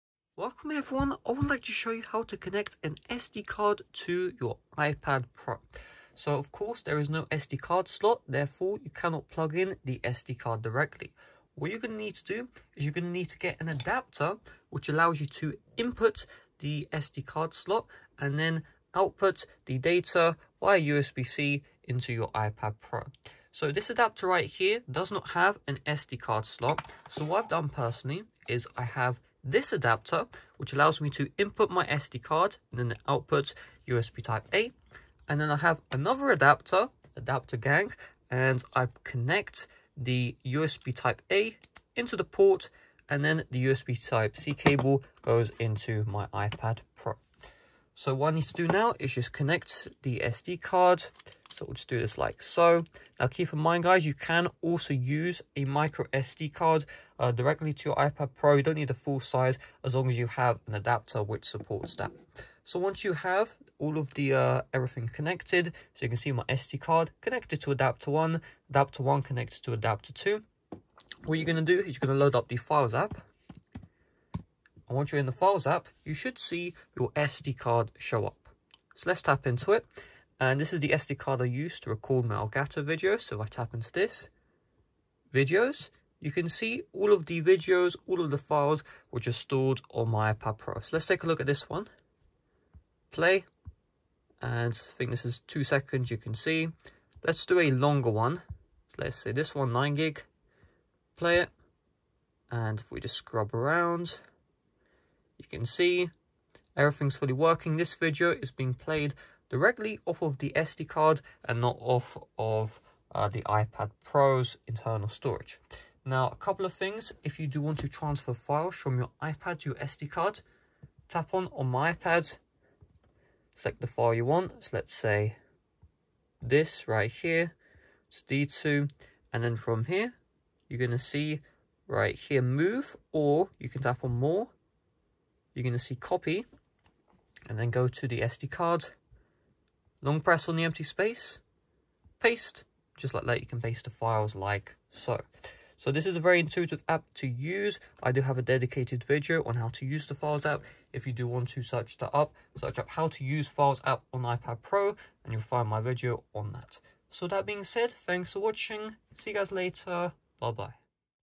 How To Connect SD Card to iPad Pro | Full Tutorial
How to connect SD card to iPad Pro | easily attach an SD card to your iPad Pro and view the files in the SD card! Do you own an SD card and want to connect it to your iPad Pro? If so, this video is for you! I will teach you how to connect a SD card to iPad Pro in this tutorial. By the end of this guide, your SD card will be connected to your iPad Pro, and you will be able to manage your SD card using the Files app.